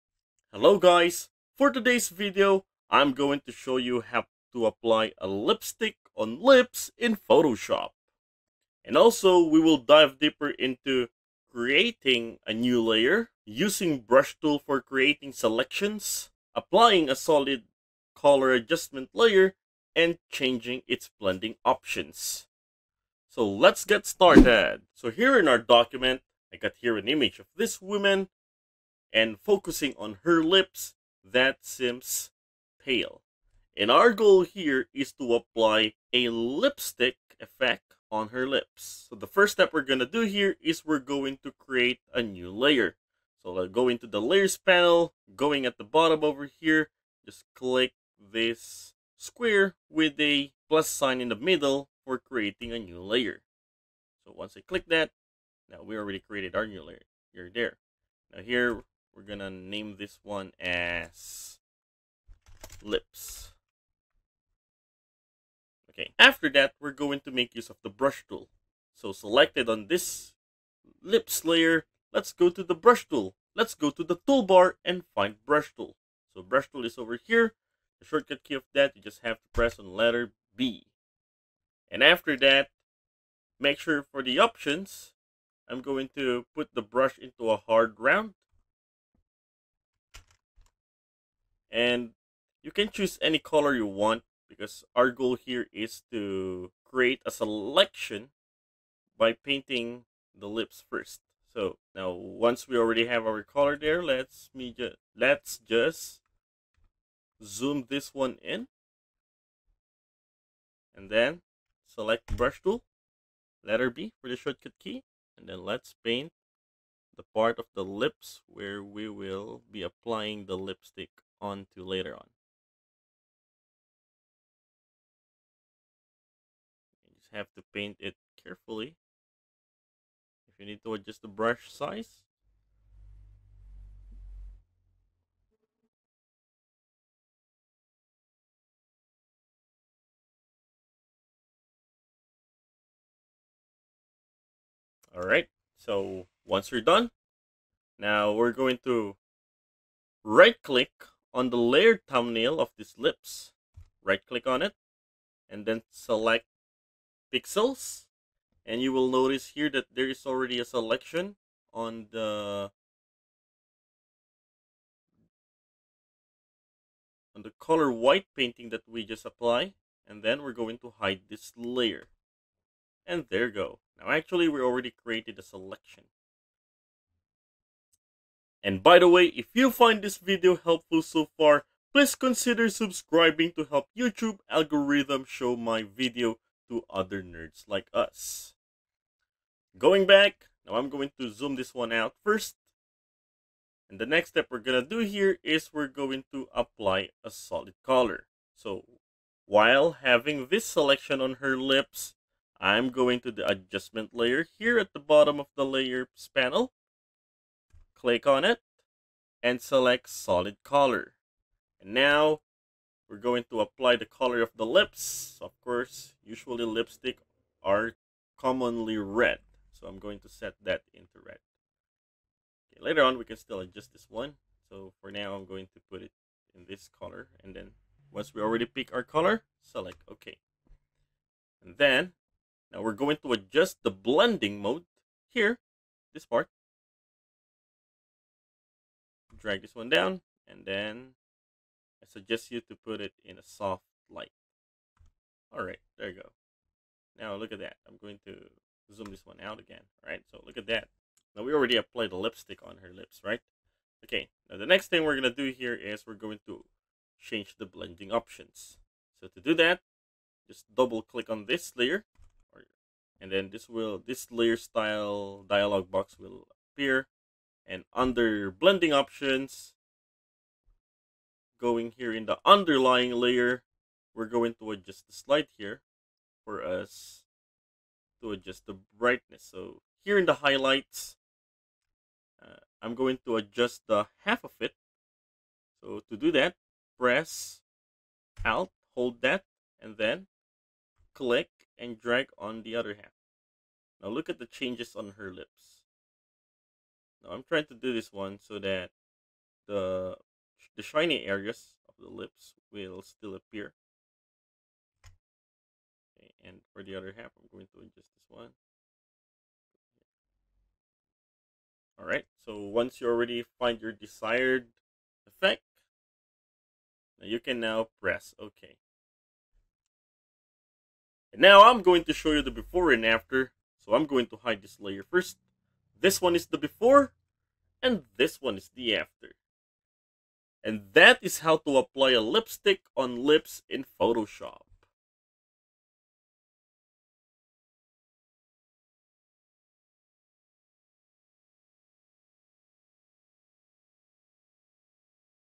How To apply A Lipstick on Lips in Photoshop
Transform your portraits by applying lipstick on lips using Photoshop!

Introduction: 00:00
Using Patch Tool: 01:12
Refining the Patch Using Clone Stamp: 02:30
Before and After: 04:10

 In this tutorial, we'll show you how to add realistic and vibrant lipstick to your photos with precision and ease. Learn step-by-step techniques to select, color, and blend lipstick shades that suit your subject perfectly. Ideal for photographers, retouchers, and digital artists looking to enhance their beauty edits. Watch now and discover how to bring your portraits to life with stunning lipstick effects!